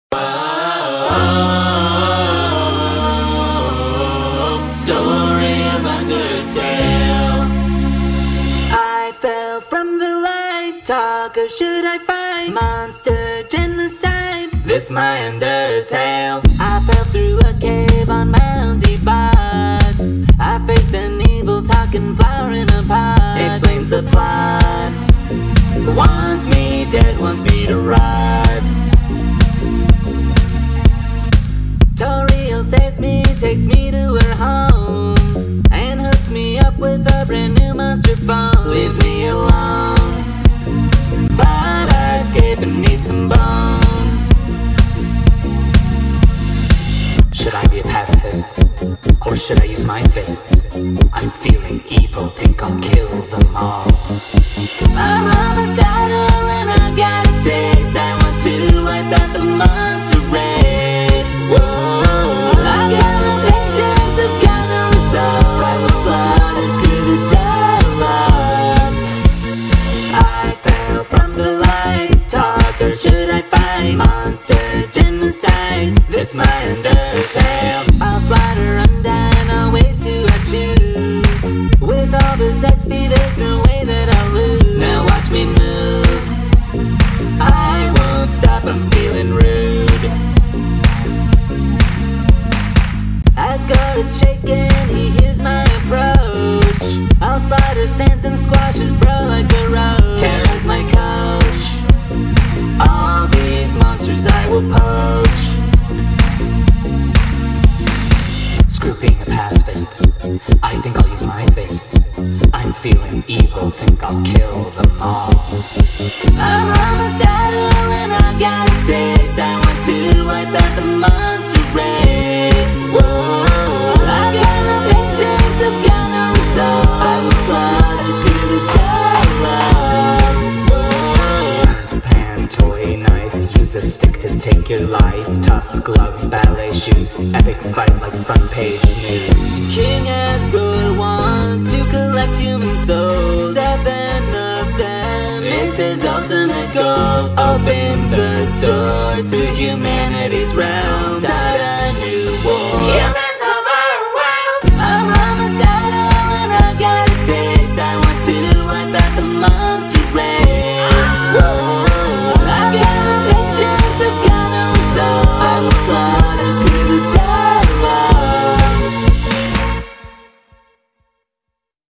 Story of undertale but it becomes character ai when it refers to a character
if you may wonder, if frisk is ai or not, it is actually a persona that I made so I can do undertale rps, tho the frisk persona I have is supposed to be pacifist one-